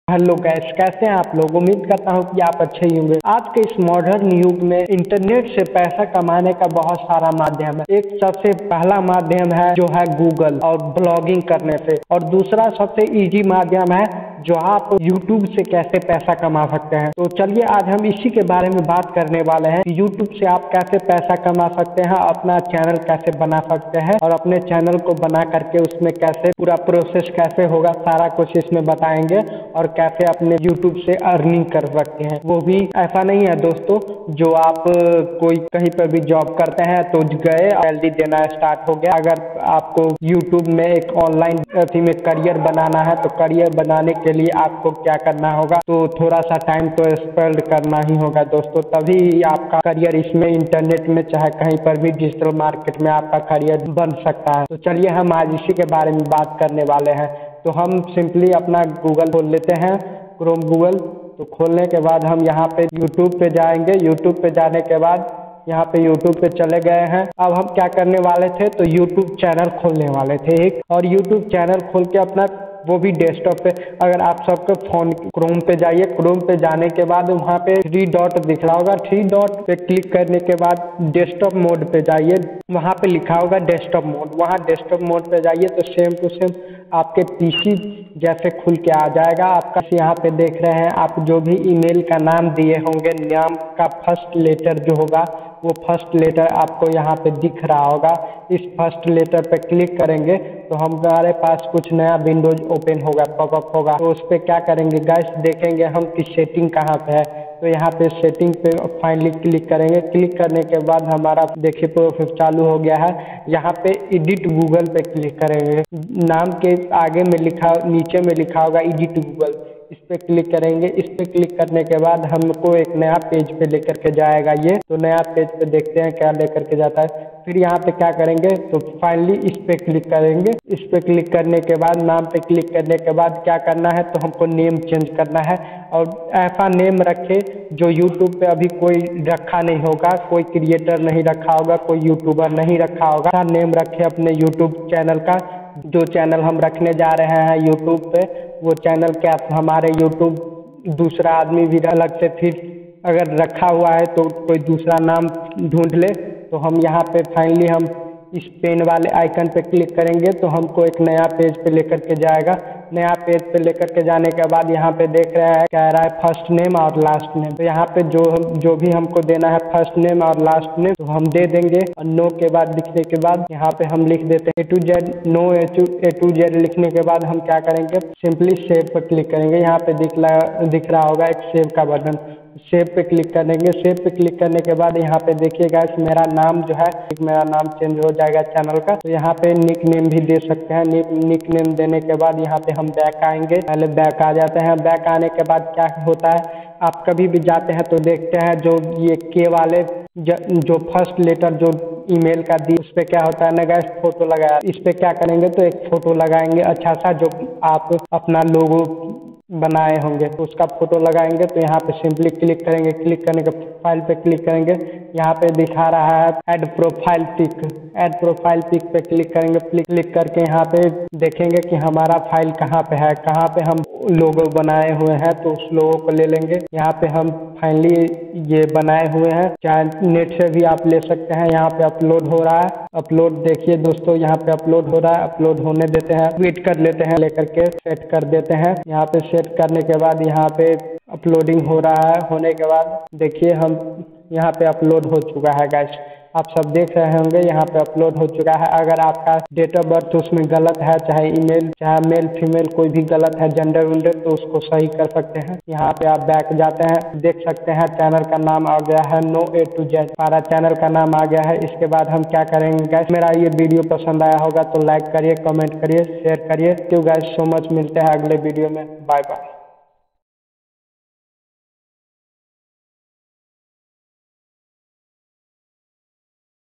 YouTube Channel Kaise BanaYe ! How to Create a New YouTube Channel,यूट्यूब चैनल कैसे बनाये Laptop🔥🔥💻
Create a channel with a business or other name
Sign in to YouTube on a computer or using the mobile site.
Go to your channel list.
Choose to create a new channel or use an existing Brand Account: ...
Fill out the details to name your new channel and verify your account.